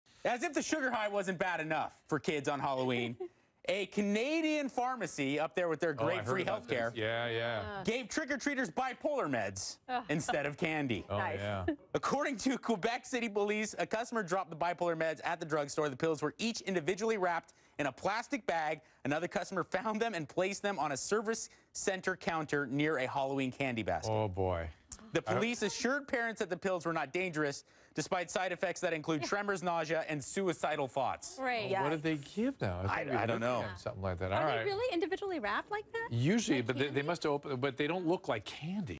Pharmacy accidentally gives out bipolar meds as Halloween candy
A worker at a Canadian pharmacy accidentally gave trick-or-treaters bipolar medication instead of candy.